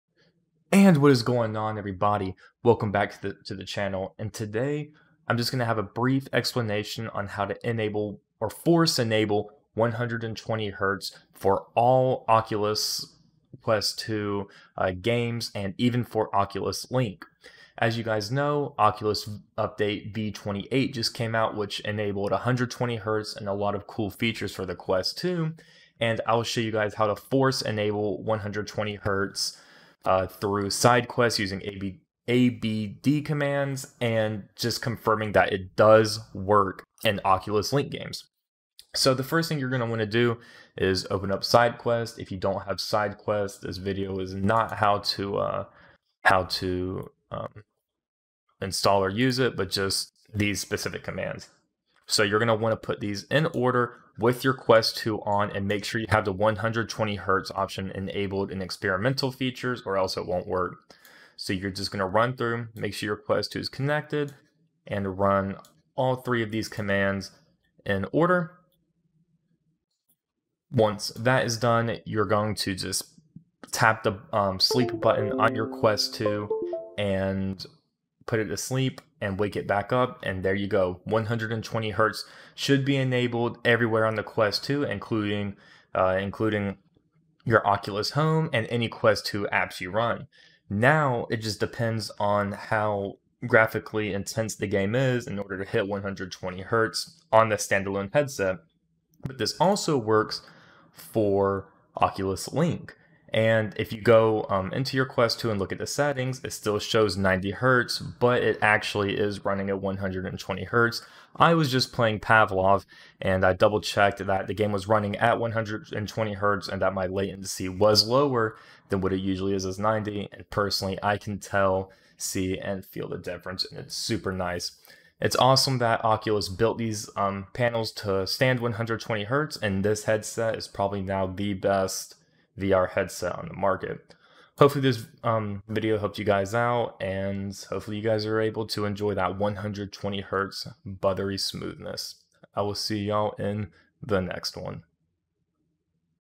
Force Enable Quest 2 120Hz! w/ SideQuest | Works In Link & Standalone! (V28)
ABD Commands for enabling 120hz on the Quest 2 (Works in Link and Standalone)

adb shell settings put global 120hz_global 1
adb shell settings put global 120hzglobal 1
adb shell setprop debug.oculus.refreshRate 120

- Snapchat  CorbinXTitus